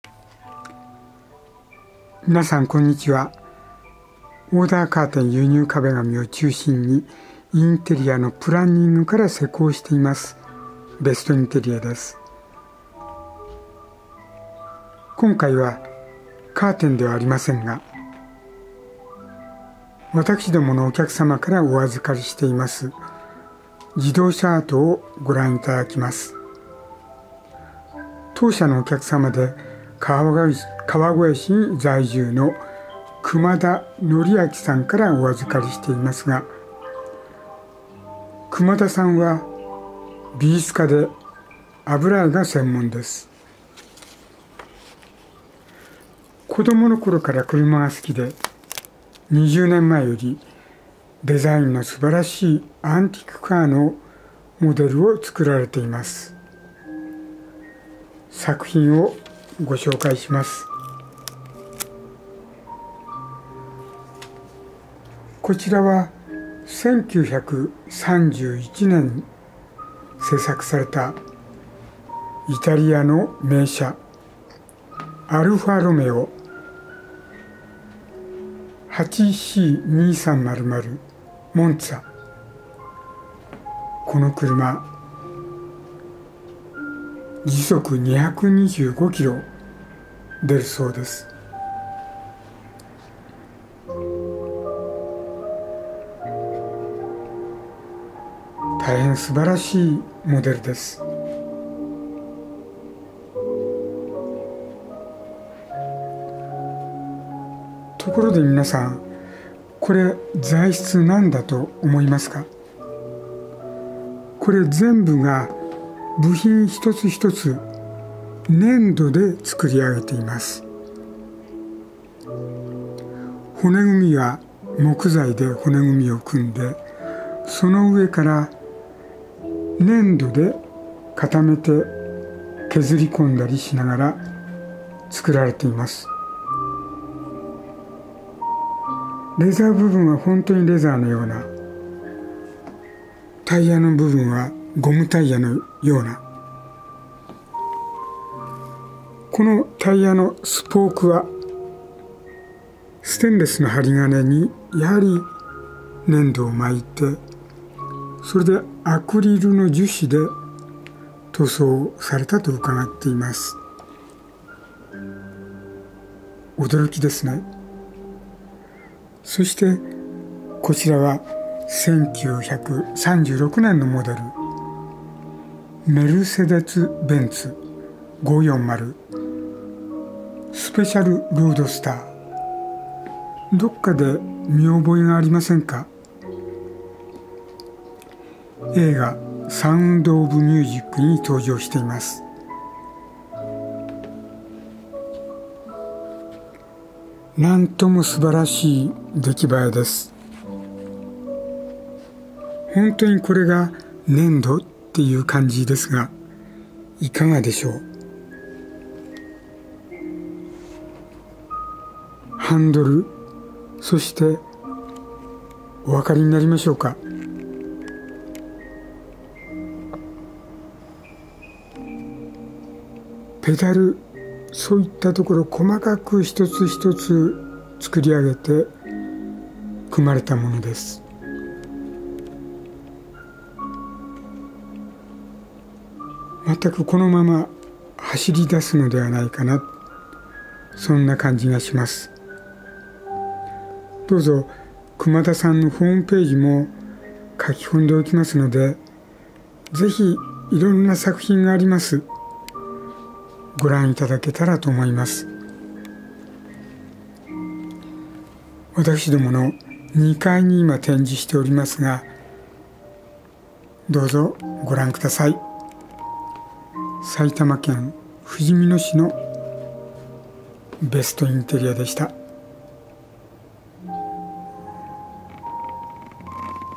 自動車アートベストインテリア粘土細工の驚きのクラシックカー
今回は当社のお客様からお預かりしている、粘土で製作の自動車アートをご紹介します。一台の製作に３か月掛かるとゆう力作です。製作者の熊田憲明さんのホームページ（HP

住まいと暮らしに夢と豊かさを提供する
株式会社ベストインテリア
埼玉県ふじみ野市ふじみ野２－１２－３１
フリーダイアル　０１２０４０５０９３